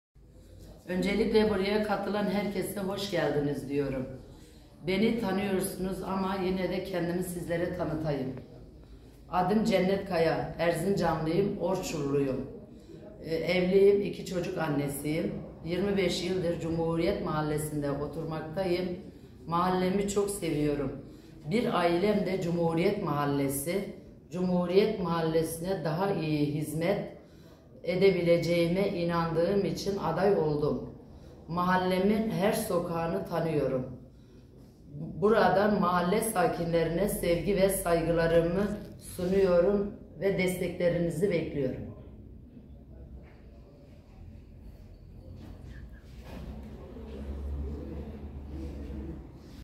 CENNET KAYA ,MUHTAR ADAYLIĞINI AÇIKLADI .
Cumhuriyet mahalle Muhtarlığı için Cenet Kaya ,adaylığını açıkladı..
Kaya'yı adaylık açıklamasında kadınlar yanlız bırakmadılar destek verdiler..